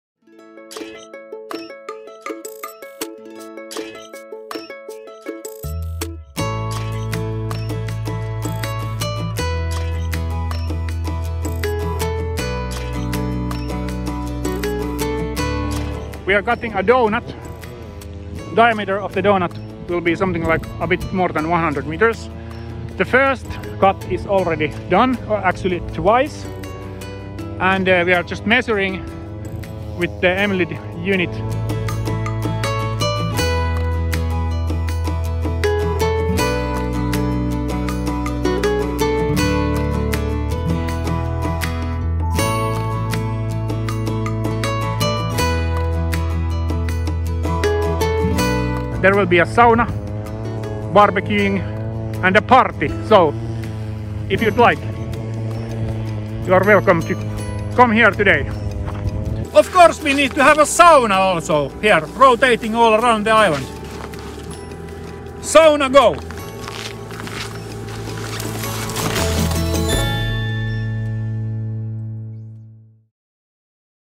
What exactly is a ‘rotating ice donut’? | AFP
A Finnish inventor has built a massive “ice donut" on a frozen lake west of Helsinki. The structure, which is 104 metres in diameter, rotates with the help of electric motors and even comes outfitted with a sauna.